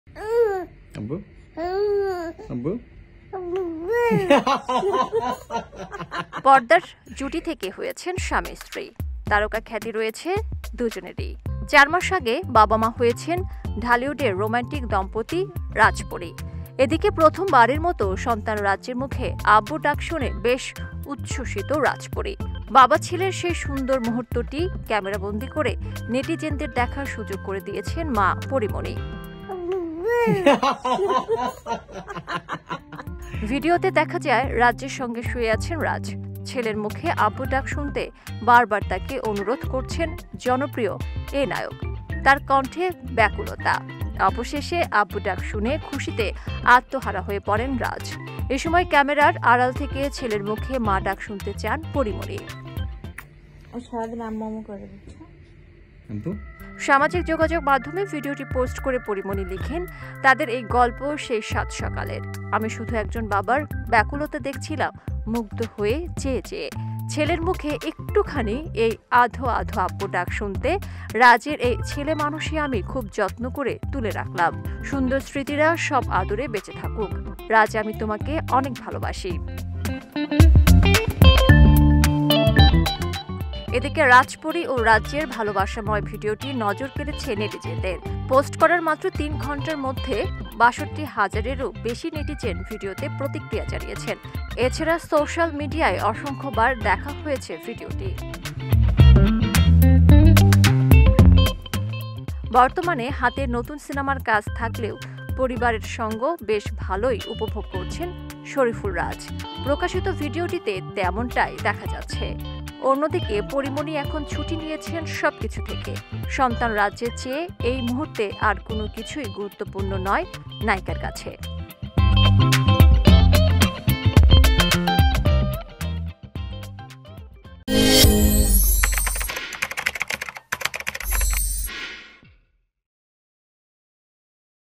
ছেলের মুখে প্রথমবার আব্বু ডাক শুনলো রাজ উচ্ছ্বসিত রাজ-পরী । Razz । Pori Moni । Bijoy TV
পর্দার জুটি থেকে হয়েছেন স্বামী-স্ত্রী। তারকা খ্যাতি রয়েছে দুজনেরই। চার মাস আগে বাবা-মা হয়েছেন, ঢালিউডের রোমান্টিক দম্পতি রাজ-পরী। এদিকে প্রথমবারের মতো সন্তান রাজ্যের মুখে ‘আব্বু’ডাক শুনে বেশ উচ্ছ্বসিত রাজ-পরী। বাবা-ছেলের সেই সুন্দর মুহূর্তটি ক্যামেরাবন্দি করে নেটিজেনদের দেখার সুযোগ করে দিয়েছেন মা পরীমণি।
ভিডিওতে দেখা যায়, রাজ্যের সঙ্গে শুয়ে আছেন রাজ। ছেলের মুখে ‘আব্বু’ডাক শুনতে বারবার তাকে অনুরোধ করছেন জনপ্রিয় এ নায়ক। তার কণ্ঠে ব্যাকুলতা। অবশেষে ‘আব্বু’ডাক শুনে খুশিতে আত্মহারা হয়ে পড়েন রাজ। এসময় ক্যামেরার আড়াল থেকে ছেলের মুখে ‘মা’ডাক শুনতে চান পরীমণি।
সামাজিক যোগাযোগমাধ্যমে ভিডিওটি পোস্ট করে পরীমণি লিখেন, তাদের এই গল্প সেই সাত সকালের! আমি শুধু একজন বাবার ব্যাকুলতা দেখছিলাম মুগ্ধ হয়ে চেয়ে চেয়ে। ছেলের মুখে একটুখানি এই আধো আধো ‘আব্বু’ডাক শুনতে রাজের এই ছেলেমানুষী আমি খুব যত্ন করে তুলে রাখলাম। সুন্দর স্মৃতিরা সব আদরে বেঁচে থাকুক। রাজ আমি তোমাকে অনেক ভালোবাসি।
এদিকে রাজ-পরী ও রাজ্যের ভালোবাসাময় ভিডিওটি নজর কেড়েছে নেটিজেনদের। পোস্ট করার মাত্র তিন ঘন্টার মধ্যে ৬২ হাজারেরও বেশি নেটিজেন ভিডিওতে প্রতিক্রিয়া জানিয়েছেন। এছাড়া সোশ্যাল মিডিয়ায় অসংখ্যবার দেখা হয়েছে ভিডিওটি।
বর্তমানে হাতে নতুন সিনেমার কাজ থাকলেও পরিবারের সঙ্গ বেশ ভালই উপভোগ করছেন শরিফুল রাজ। প্রকাশিত ভিডিওটিতে তেমনটাই দেখা যাচ্ছে। অন্যদিকে পরীমণি এখন ছুটি নিয়েছেন সব কিছু থেকে। সন্তান রাজ্যর চেয়ে এই মুহূর্তে আর কোন কিছুই গুরুত্বপূর্ণ নয় নায়িকার কাছে।